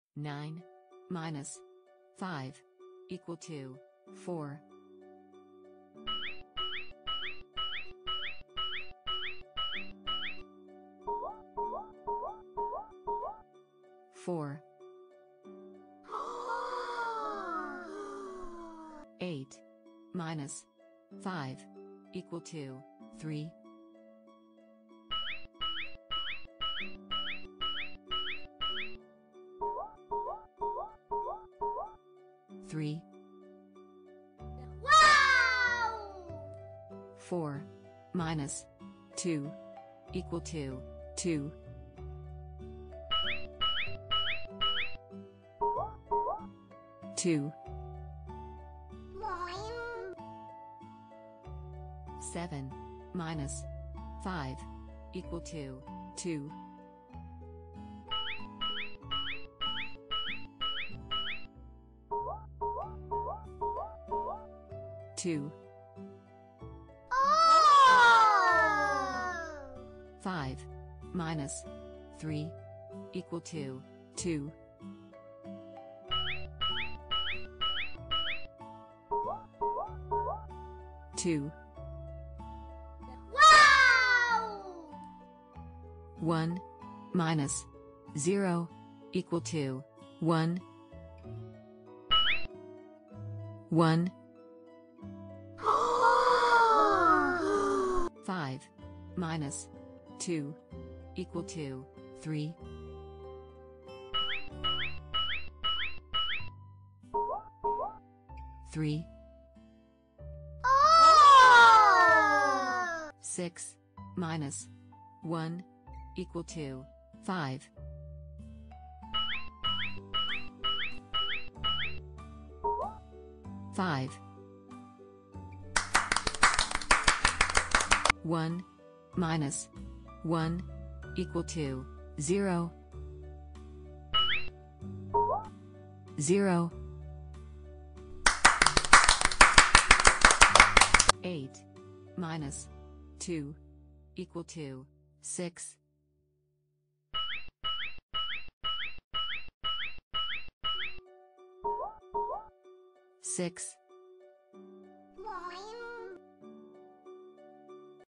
Learning to subtract for children
Educational video to learn to subtract for children in English